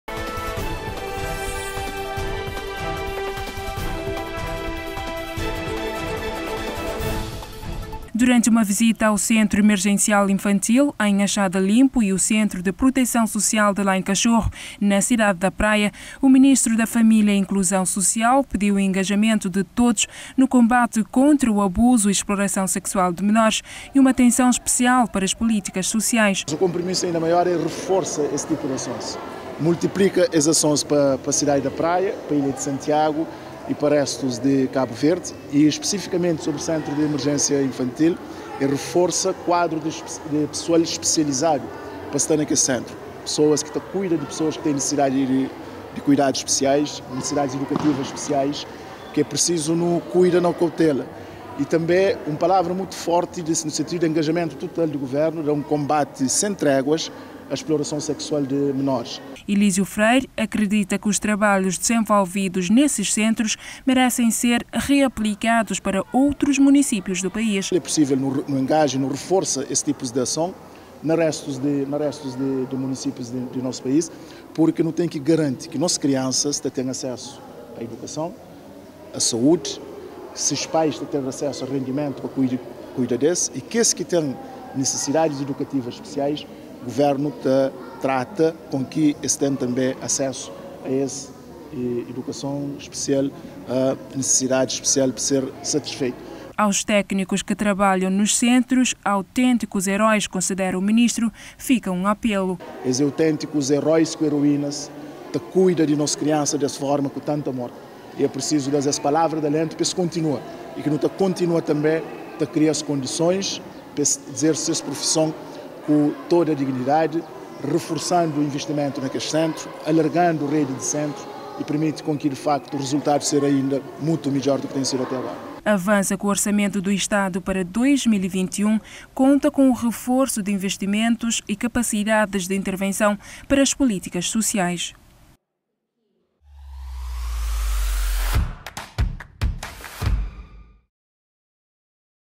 CENTRO DE PROTEÇÃO/EMERGENCIAL INFANTIL
Governo considera que é preciso um reforço da classe especializada que cuida das crianças em situação de risco e com necessidades educativas especiais.

👇 Acompanhe também👇📌